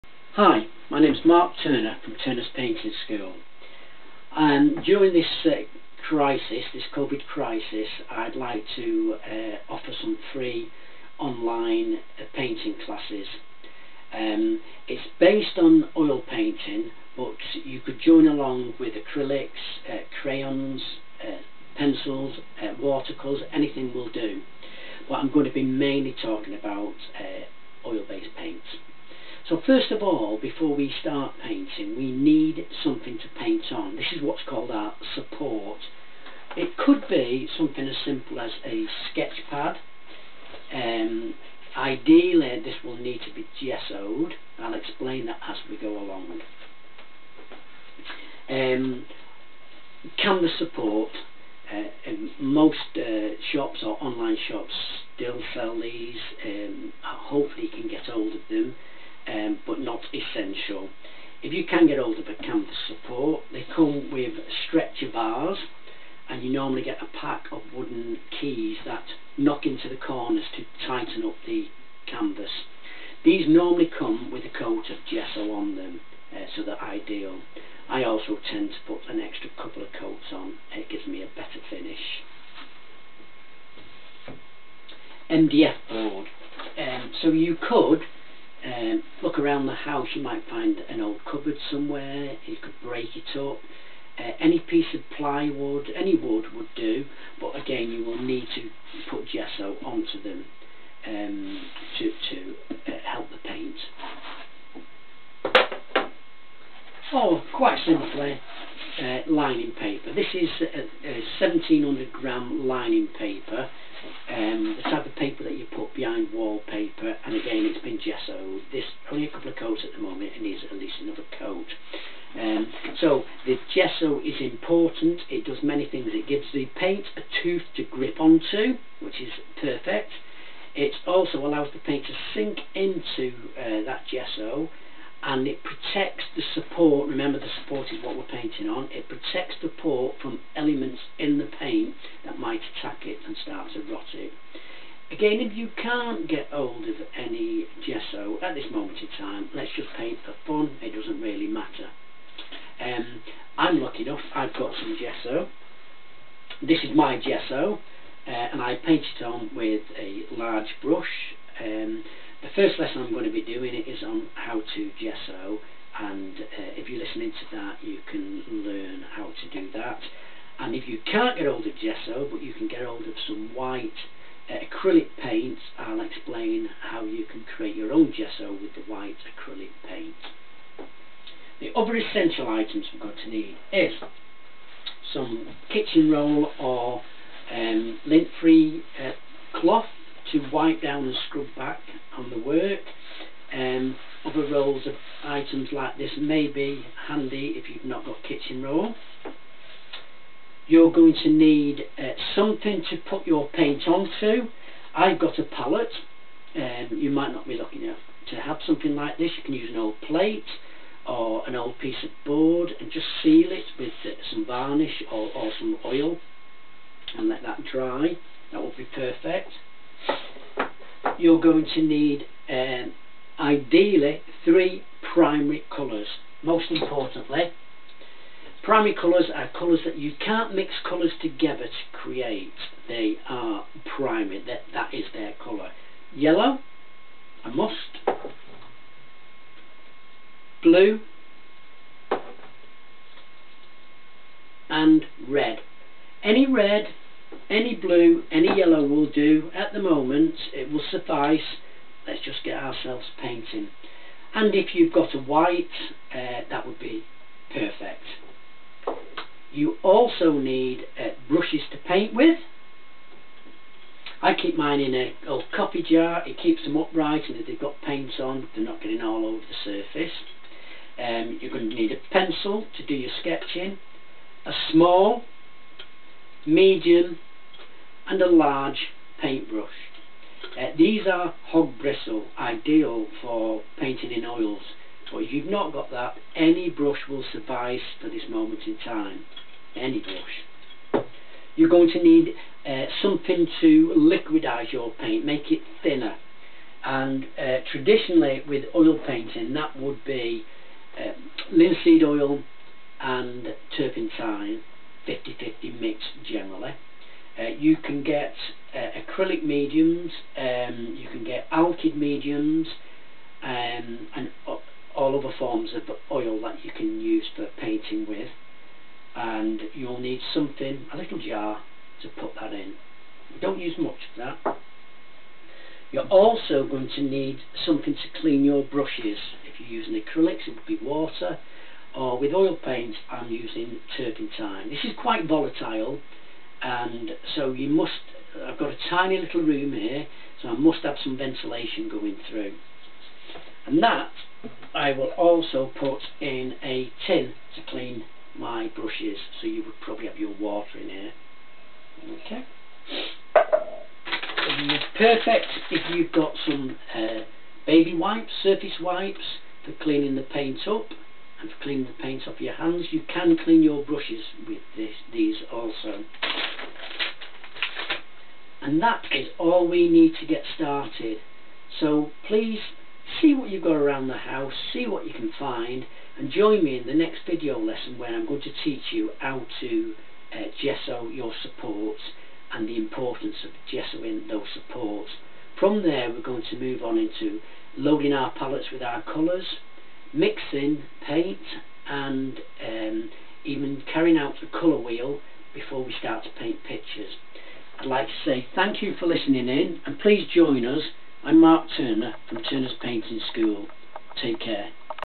Free Introduction to Oil painting
in these difficult times, Marc A Turner and Turners Painting School would like to offer Free painting tuition to help over lock down, the classes will be short and simple so that you can look and learn something along the way.